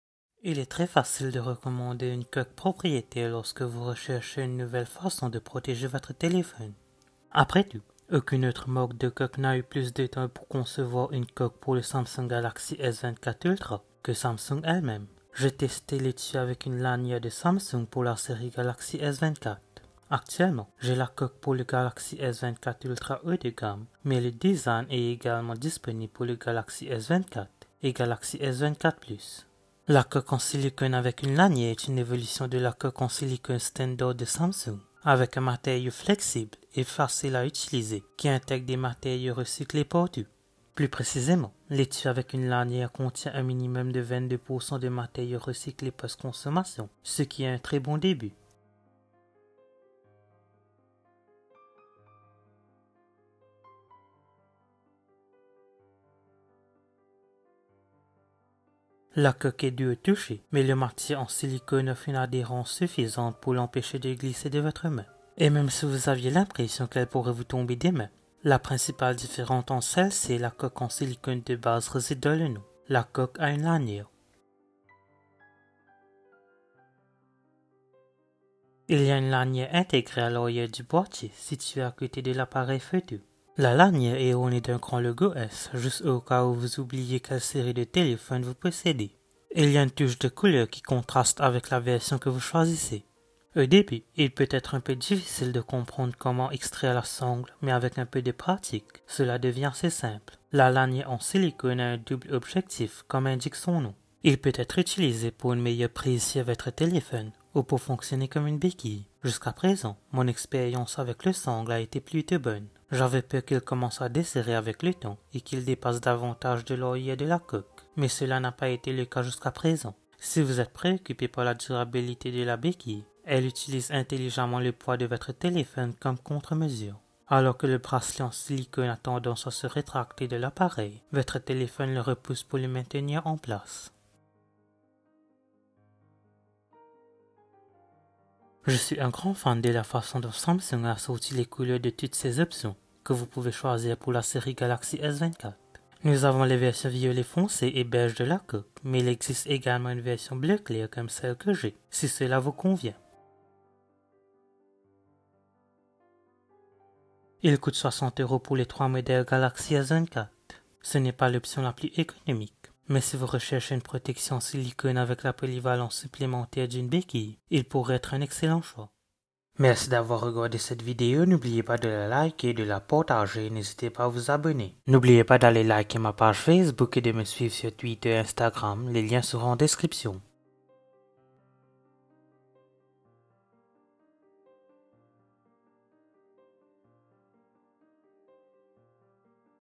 Coques officielles du Samsung Galaxy S24 Ultra: Faut-il craquer pour ces accessoires haut de gamme ?
Il est très facile de recommander une coque propriétaire lorsque vous recherchez une nouvelle façon de protéger votre téléphone.  Après tout, aucune autre marque de coques n'a eu plus de temps pour concevoir une coque pour le Samsung Galaxy S24 Ultra que Samsung elle-même.  J'ai testé l'étui avec lanière de Samsung pour la série Galaxy S24.

 Actuellement, j'ai la coque pour le Galaxy S24 Ultra haut de gamme, mais le même design est également disponible pour les Galaxy S24 et S24 Plus.  La coque en silicone avec une lanière est une évolution de la coque en silicone standard de Samsung, avec un matériau flexible et facile à utiliser qui intègre des matériaux recyclés partout.  Plus précisément, l'étui avec une lanière contient un minimum de 22 % de matériaux recyclés post-consommation, ce qui est un très bon début.

Vero

Bereal

Mastodon

Blue Sky

Time Code:

00:00 Intro
00:55 Le touché
01:14 l’arrière
02:14 Couleur
02:33 Conclusion
02:49 Outro

Video Link:    / @lamusiquelibre